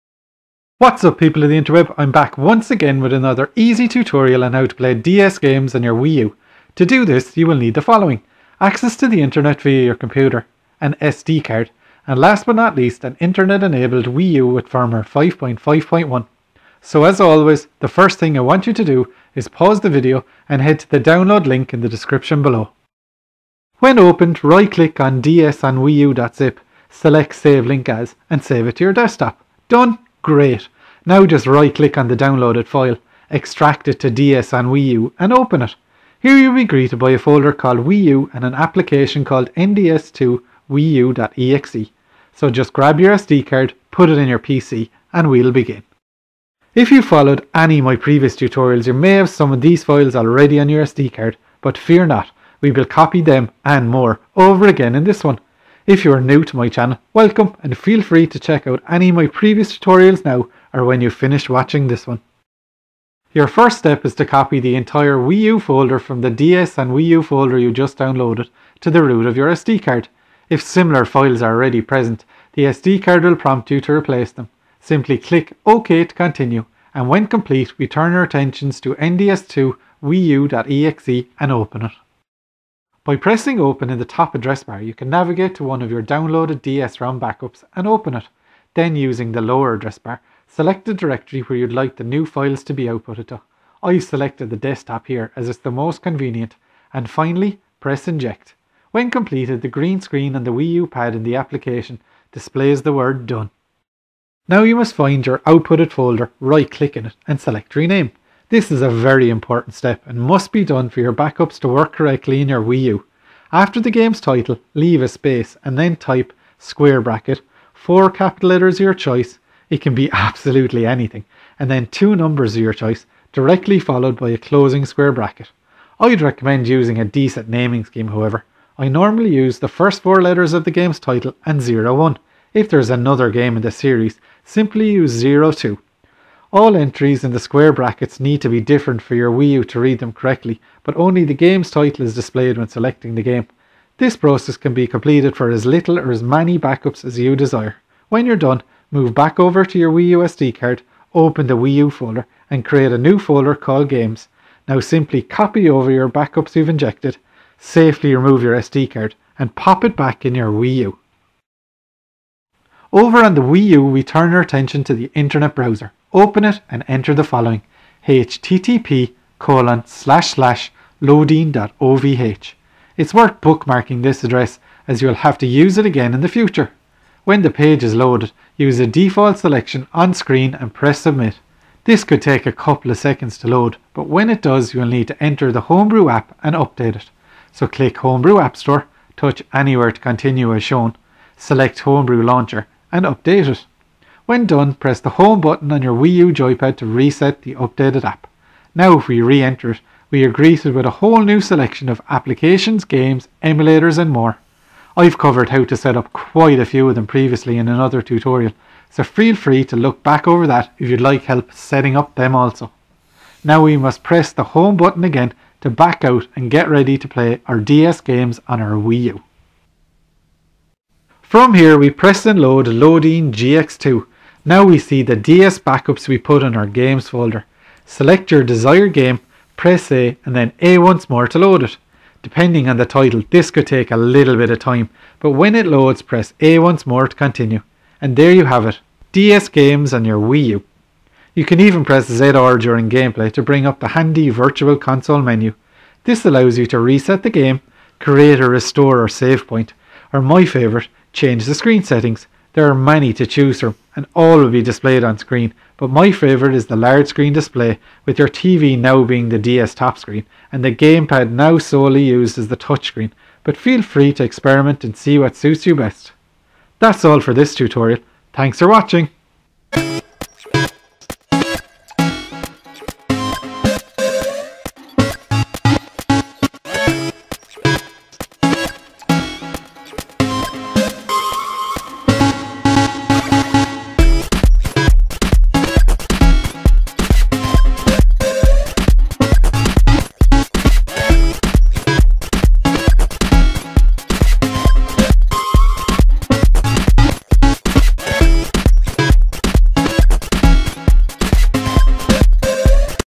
Easy Wii U 5.5.1 Mod/Hack - Playing DS Games On Your Wii U
A quick, easy and effective tutorial showing you from start to finish how to play DS games on your Wii U

Important Notes:

*This tutorial is for instructional purposes only to show you what can be done! Only install backups of games you actually own!*

*This is a soft mod and can be reversed!*

*Loadiine is a web based access tool for Homebrew Launcher and because of the nature of this it will hang or freeze from time to time. Simply rebooting the system normally fixes this!*

*If that doesn't work a couple of times in a row the browser has probably cached the error. Remove the SD card. Try loading Loadiine again and then accept there will be an error! Reboot the Wii U. Insert the SD card and try again! This works 99% of the time if step two hasn't worked!*

*If either of the above two steps hasn't worked its probably an error due to Loadiine being updated! It's a web based tool that is constantly being updated to improve it! Bare with it and try again later!*

Links to My Other Wii U Tutorials